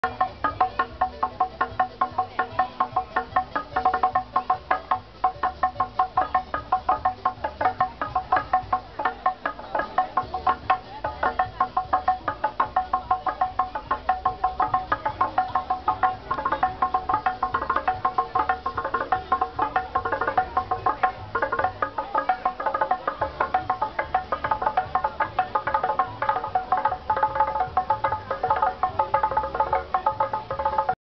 San Pedro Zarikete 2012: Txalaparta
Txalapartariak San Pedro Zariketeko jaiak goizetik alaitzen (Zalla)
Txalapartaris animando las fiestas de San Pedro Zarikete (Zalla)